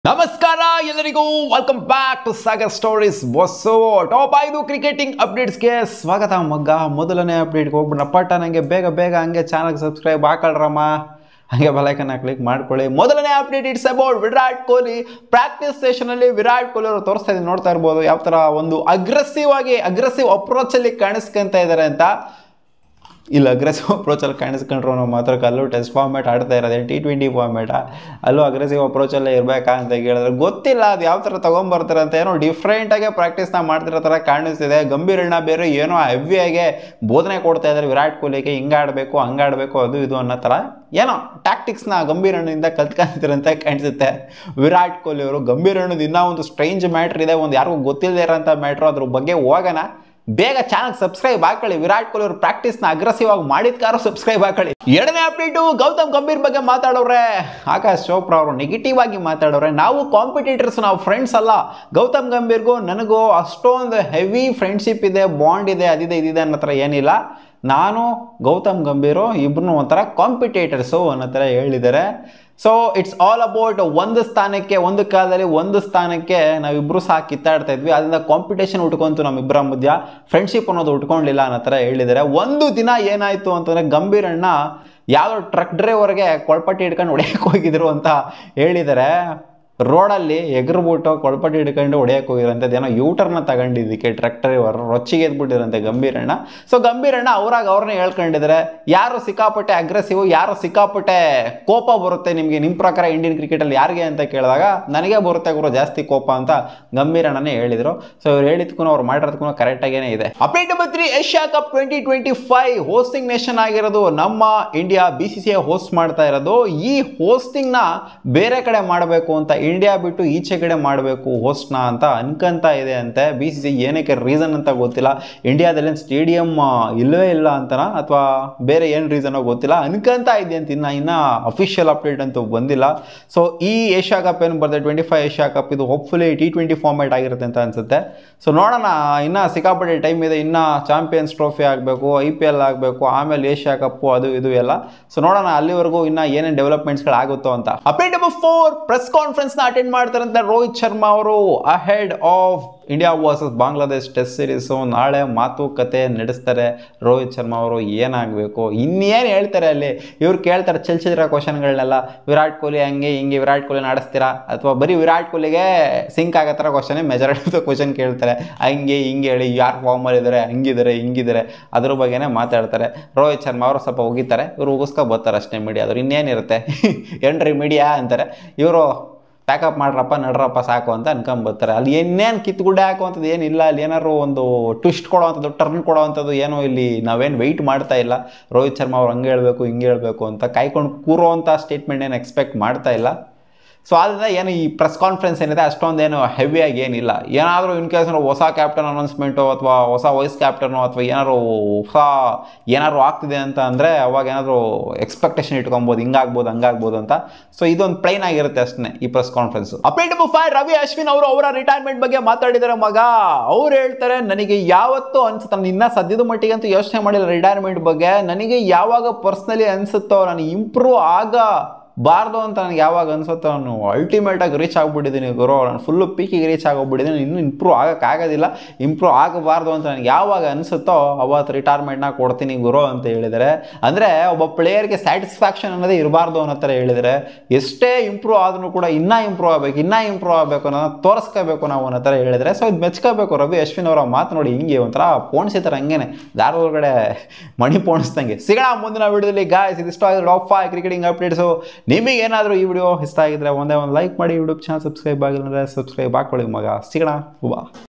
Virat Kohli intense practice ahead of IND VS BAN test series|Gautam Gambhir VS Aakash Chopra
Hi elrigu namskara